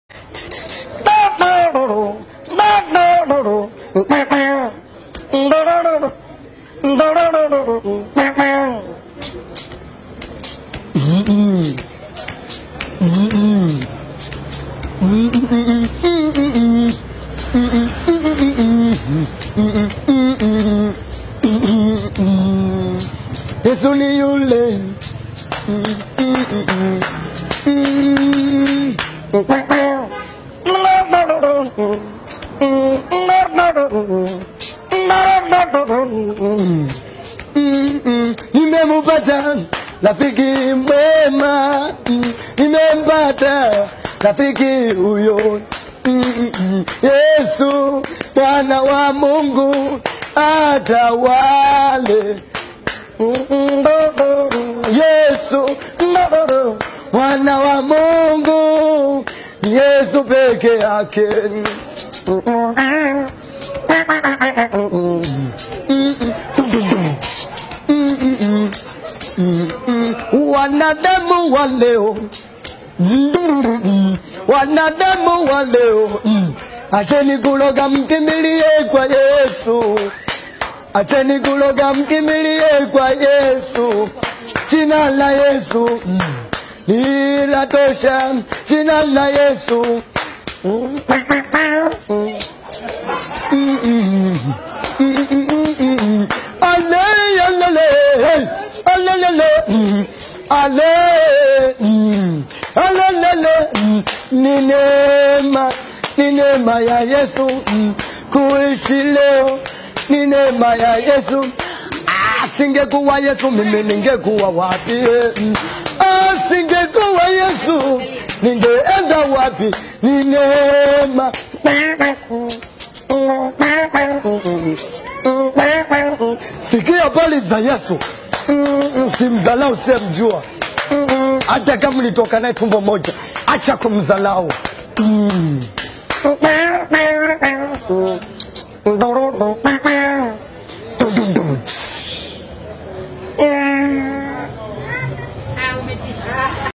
unique talent
The guy can sing and produce instrumental sounds using his body.